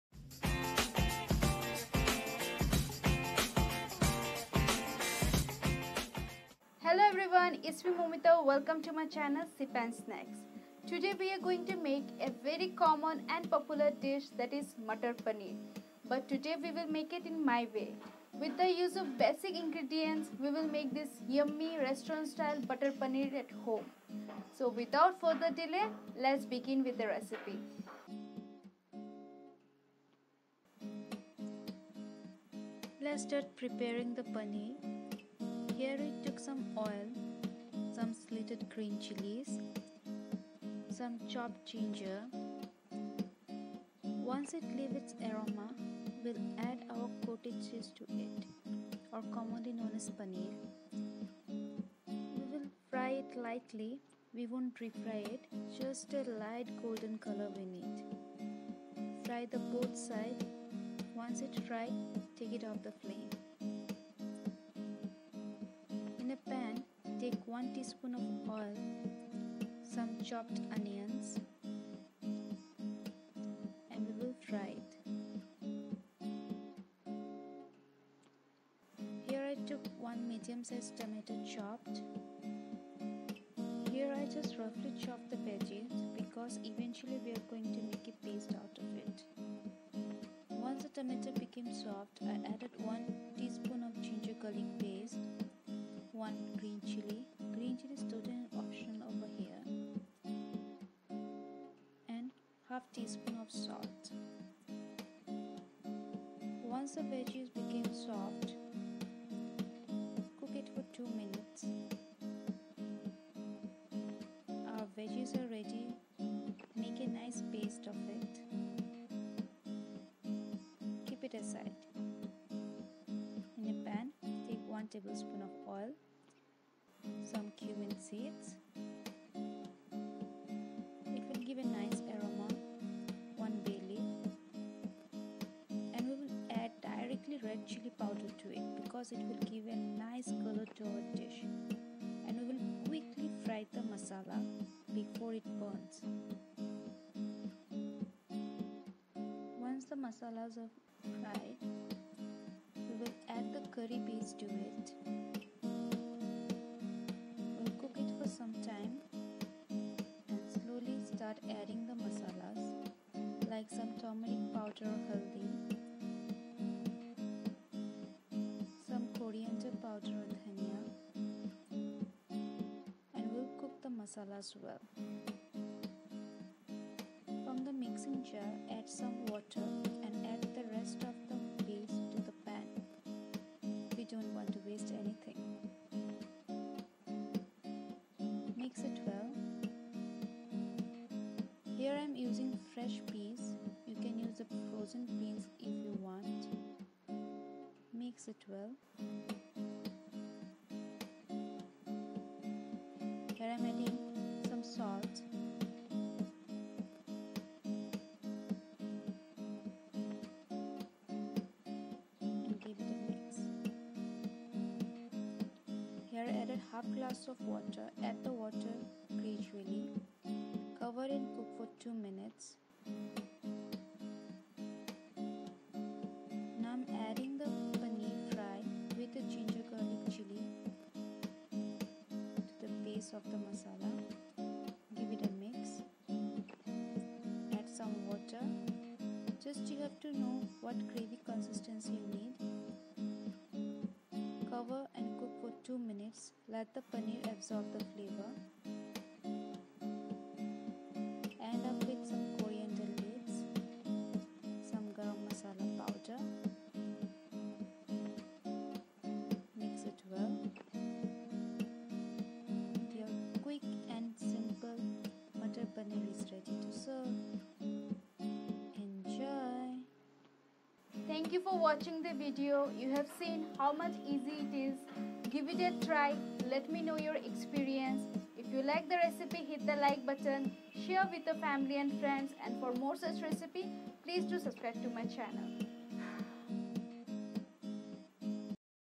Dhaba Style Matar Paneer Recipe at Home || Paneer Curry
Dhaba Style "Matar Paneer" recipe at home. Check out my way of simple and quick paneer (cottage cheese) with matar(fresh peas) recipe. Most popular Indian Dish.

Ingredients –

*Paneer
*Matar
*Ginger Garlic Paste
*Salt
*Red Chilli Powder
*Turmeric Powder
*Cumin Powder
*Coriander Leaves
*Onion
*Garam Masala Powder
*Tomato
*Oil